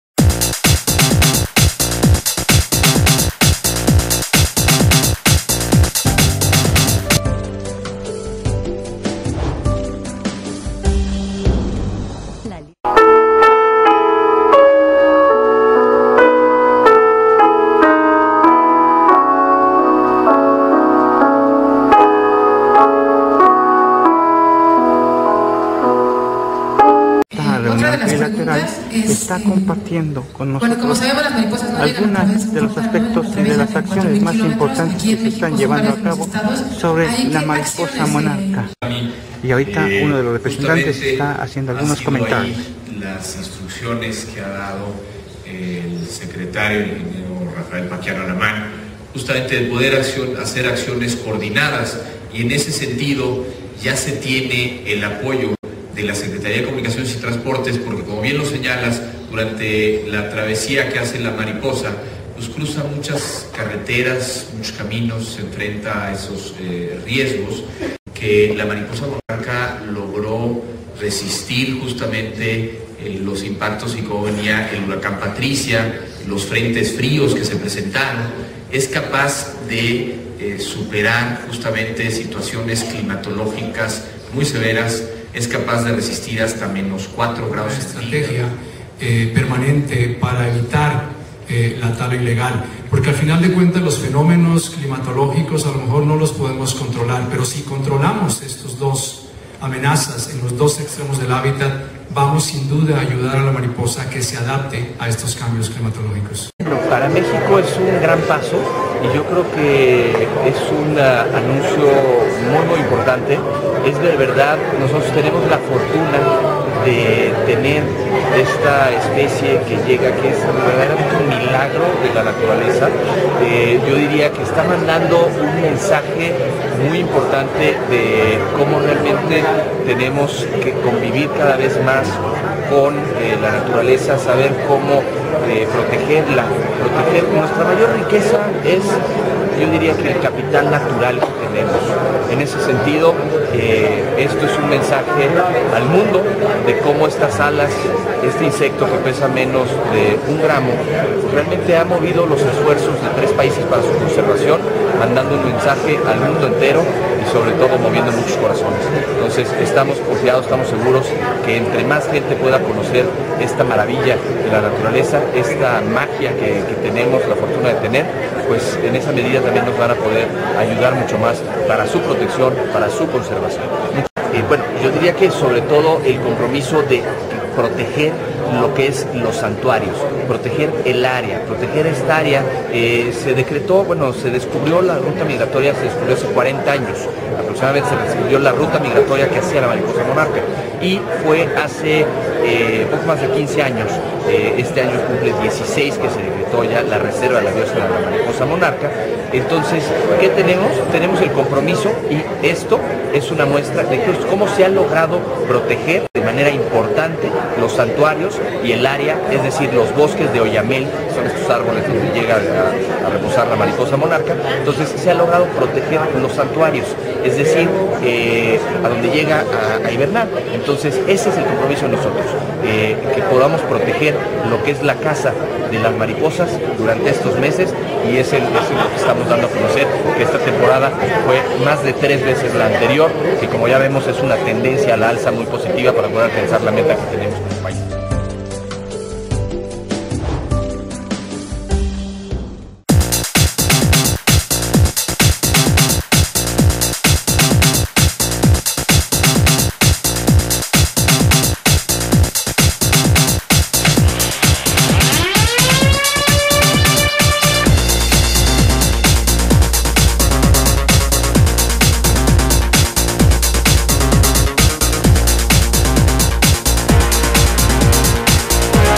El territorio que ocupó este año la mariposa monarca creció tres veces más
La superficie ocupada por la mariposa Monarca en los bosques de pino y oyamel en los santuarios ubicados en los estados de México y Michoacán registró la cifra de 4.01 hectáreas,  tres veces más que la temporada pasada cuando se registraron 1.13 hectáreas.
De acuerdo con el monitoreo realizado por la Comisión Nacional de Áreas Naturales Protegidas (CONANP), en coordinación con el Fondo Mundial para la Naturaleza (WWF por sus siglas en inglés), esta superficie representa un incremento de 255 por ciento en relación  a la tenida en  diciembre del 2014 de 1.13 has.
En conferencia de prensa,  Alejandro Del Mazo Maza, Comisionado Nacional de Áreas Naturales Protegidas de México,  acompañado  de Dan Ashe, Director del Servicio de Pesca y Vida Silvestre de Estados Unidos, Gilles Seutin, científico  especialista en  Ecosistemas del  Departamento de Parques de Canadá, y  representante del Gobierno de Canadá y la bióloga Gloria Tavera,  directora Regional Centro y Eje Neovolcánico
Los representantes de los tres países informaron sentirse motivados y sorprendidos por los resultados obtenidos en esta temporada y señalaron que este resultado representa un gran avance para alcanzar la meta establecida trilateralmente de 6 hectáreas de superficie de bosque para el  2020, y es pauta para continuar trabajando en favor de esta especie emblemática de la región.
Alejandro Del Mazo señaló que la cifra dada a conocer marca una tendencia ascendente, lo cual fortalece la estrecha colaboración con Canadá y Estados Unidos  para consolidar estos esfuerzos comunes.